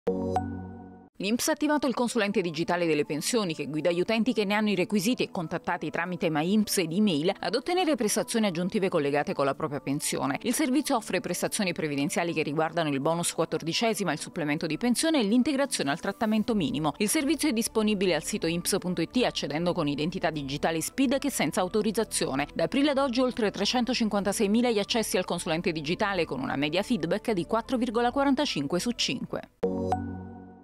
Pensioni, come funziona il Consulente Digitale INPS
Tra i nuovi servizi INPS il Consulente Digitale delle Pensioni
permette ai cittadini di verificare se hanno diritto a un'integrazione pensionistica e li accompagna nel percorso per ottenerla.